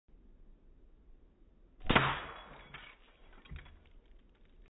Slow motion bottle breaking (part 2)
Shooting glass bottle with BB gun